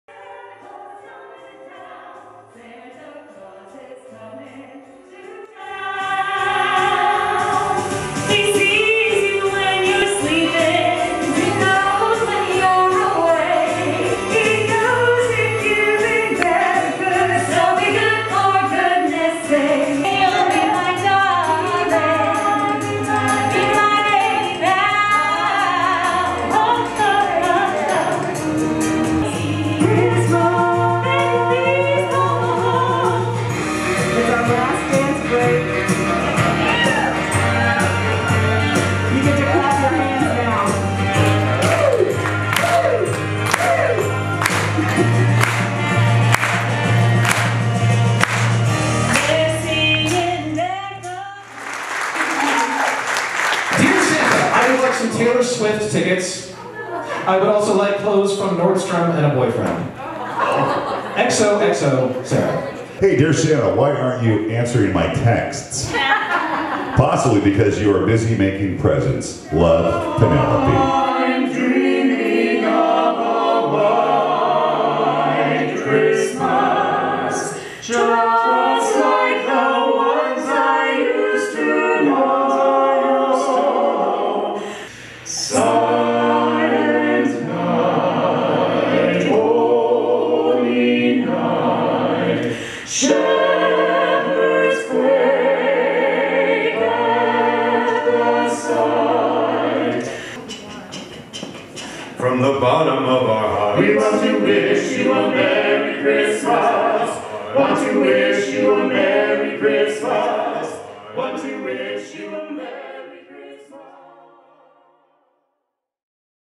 Marysville Holiday Variety Show 2021
A recap of our festive Holiday Variety Show featuring Stitch Mitchell, The Jewel Tones and Premier Vocal Entertainment's Holiday Harmony Carolers.

Wishing you and yours a happy holiday season!